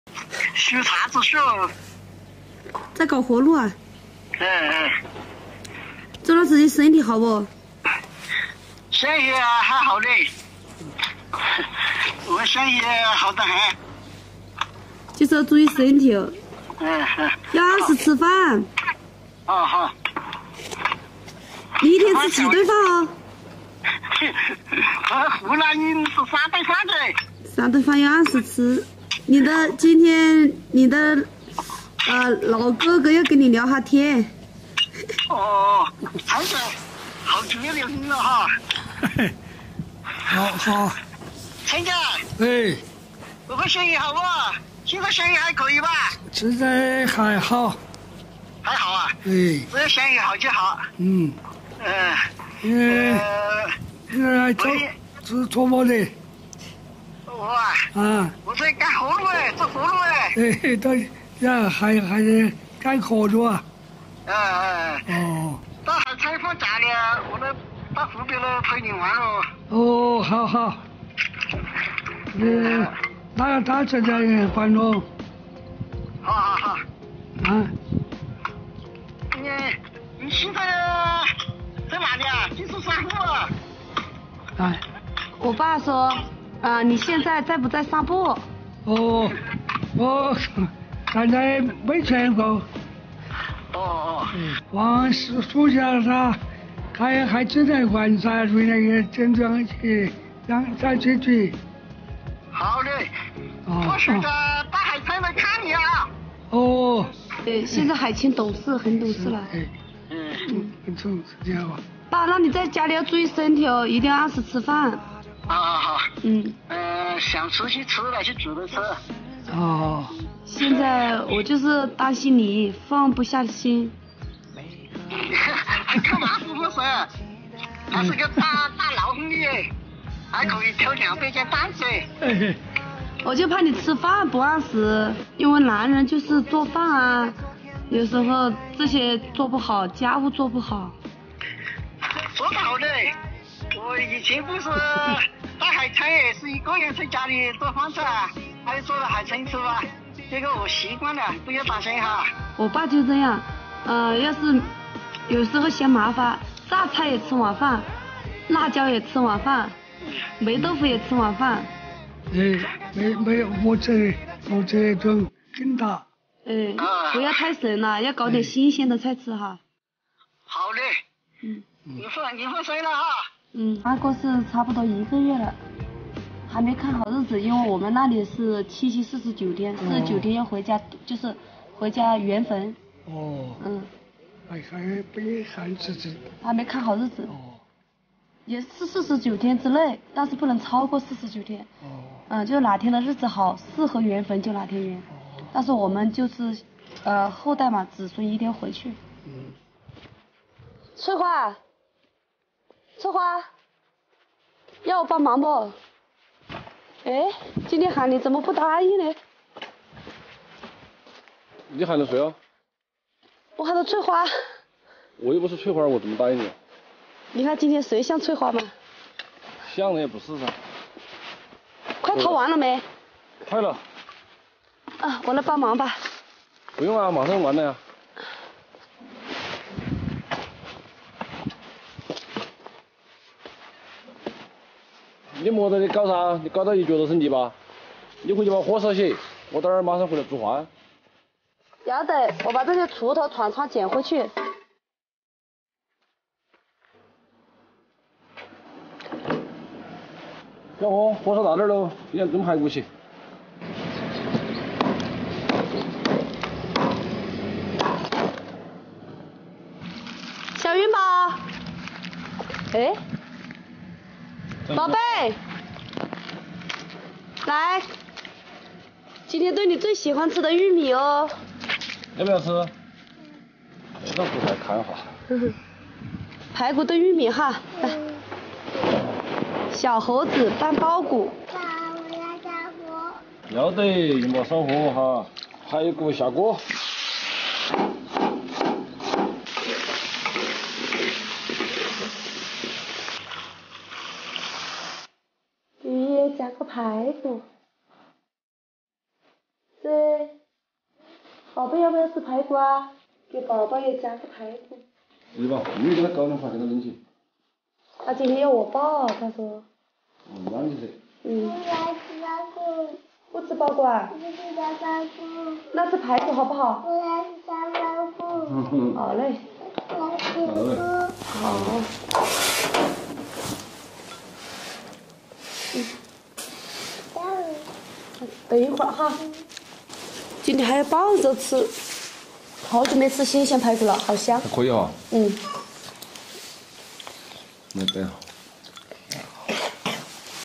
岳父一个人在家孤苦伶仃，小凤很担心，放假决定接海青岳父来湖北
油管的观众们大家好
我是土家小木
分享最淳朴的农村生活
感受最放松的日常氛围
感恩认识的每一个朋友！